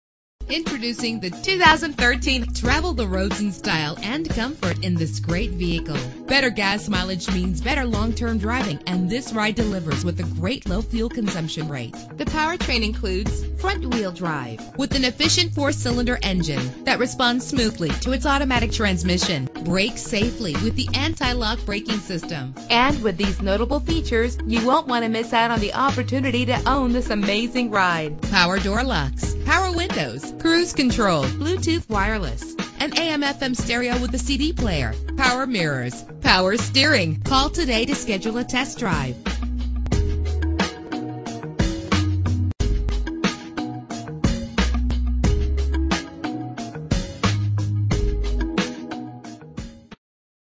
2013 Toyota Prius v - Wichita KS
This vehicle represents similar vehicles available at Eddy's Toyota.
Year: 2013
Make: Toyota
Model: Prius v
Trim: Three
Engine: 1.8 liter inline 4 cylinder 16 valve MPFI DOHC
Transmission: Automatic CVT
Color: Magnetic Gray Metallic
Mileage: 6
Address:
7333 E Kellogg Dr